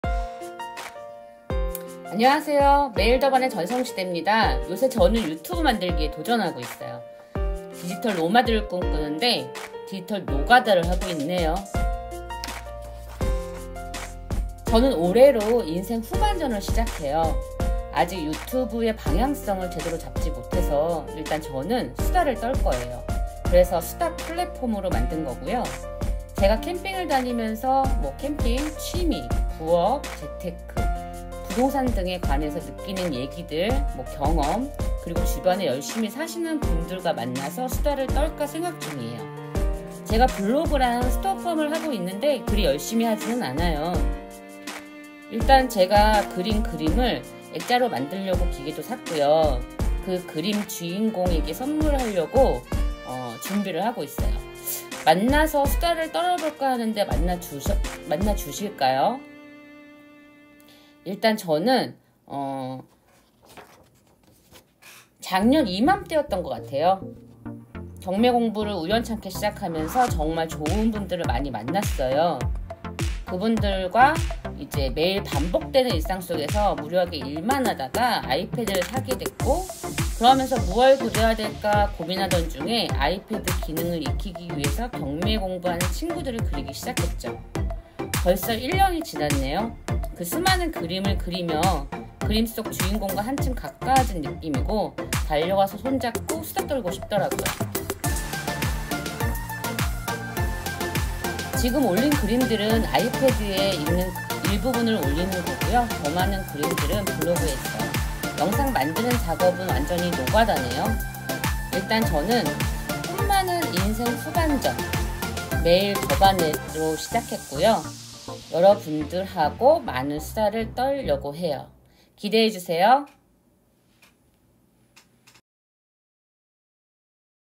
[돈버는 취미]취미로 사진 일러스트 그림 액자만들기 '매일더반해' 전성시대의 수다가 시작됩니다
그림을 그리며 수다떠는 전성시대의 꼼지락공장 '매일더반해'
디지털노마드를 꿈꾸는 디지털노가다세상
한걸음내디어 봅니다

00:00 전성시대입니다
00:16 수다플랫폼 시작
00:47 그림주인공들
01:48 매일더반해